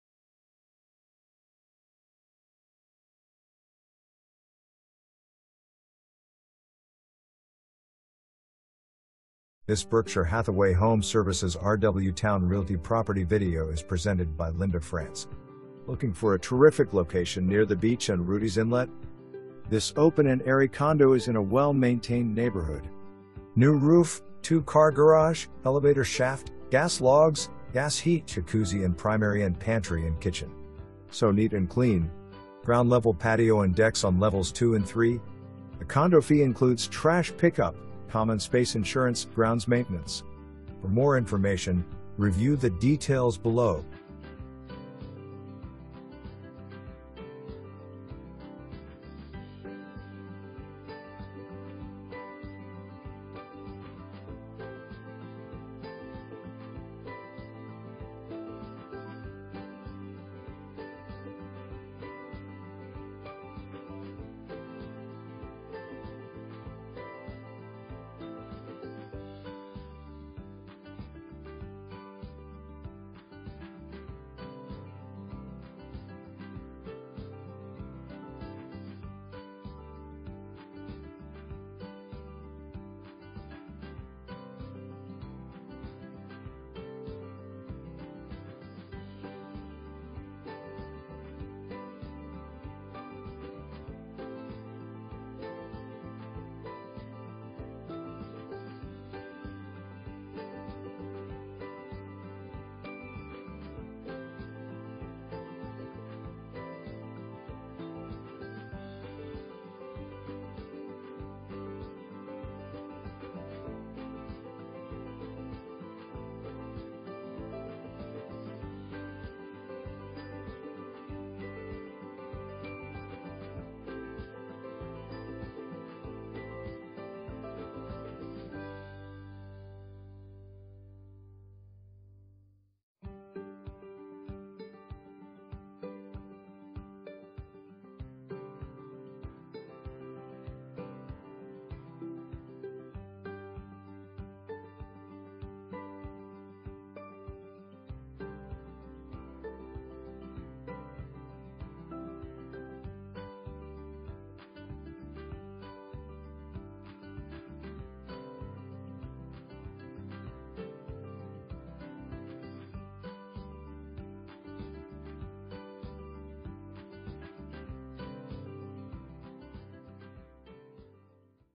Homes for Sale - 708 Sequoia WAY, Virginia Beach, VA
Looking for a terrific location near the beach and Rudee's Inlet? This open and airy condo is in a well-maintained neighborhood. New roof, two car garage, elevator shaft, gas logs, gas heat, jacuzzi in primary & pantry in kitchen. So neat and clean! Ground-level patio and decks on levels 2 & 3. The condo fee includes trash pickup, common space insurance, grounds maintenance.

2 full bath, 1 half bath

Linda Frantz
Berkshire Hathaway HomeServices RW Towne Realty